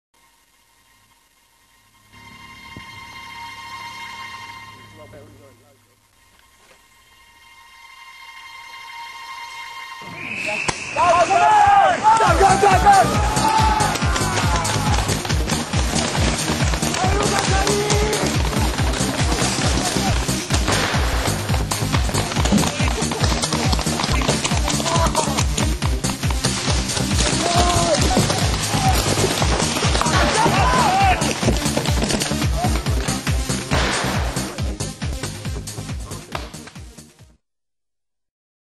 Skirmish Paintball Exeter Devon Paintball Exeter
Skirmish Exeter is set in one hundred acres of woodland, forming part of a six hundred acre forest called Stoke Woods. It is situated just five minutes outside of Exeter and is easily found whether you are travelling from the M5, A30, or A303. There are eighteen seperate game areas, providing unique combinations of natural and man made cover.